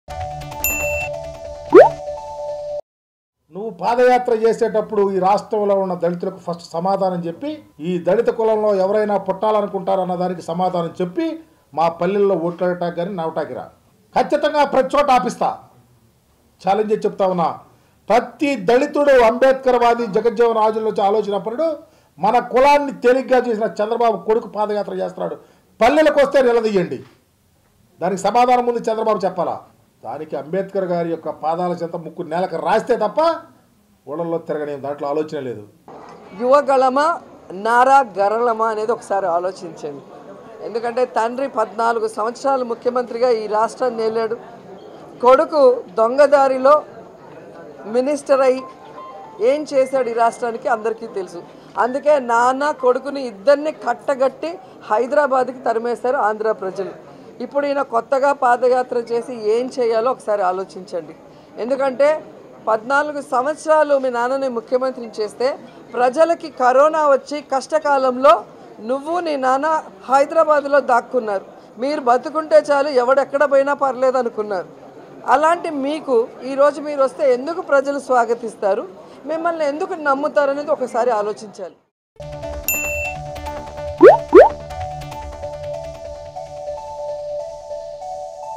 చంద్రబాబు దళితులపై చేసిన వ్యాఖ్యలకు లోకేష్ సమాధానం చెప్పి పాదయాత్రకు చేయి ||YCP Ministers on Lokesh
చంద్రబాబు దళితులపై చేసిన వ్యాఖ్యలకు లోకేష్ సమాధానం చెప్పి పాదయాత్రకు చేయి
YCP Ministers on Lokesh
YCP Ministers fire on Nara Lokesh